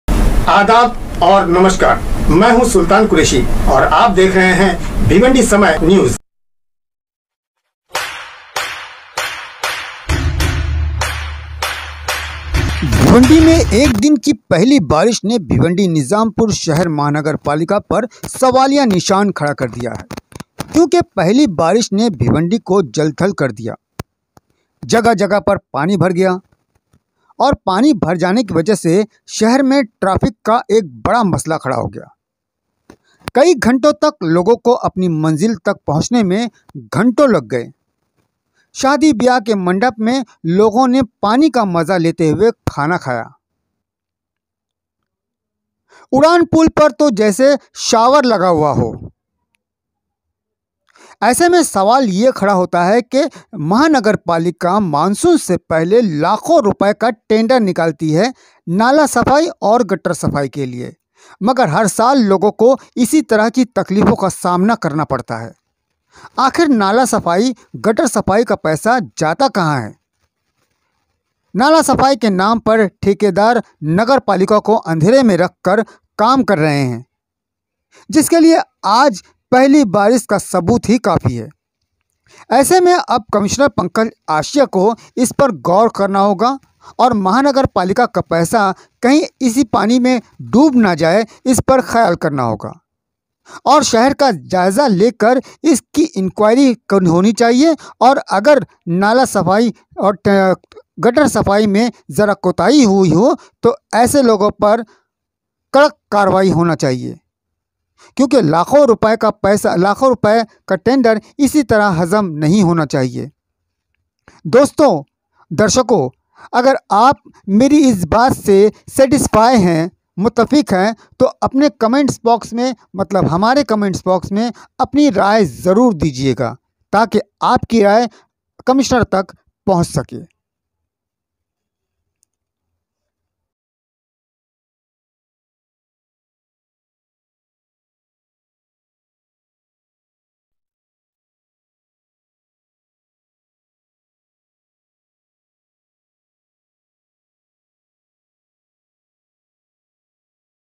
साहेब भिवंडी डूब गई
भिवंडी की पहेली बारिश ने पूरे शहर को जल थल कर दिया और हर साल की तरह नाला सफाई के नाम पर धन उगाही करने वालो पर सवालिया निशान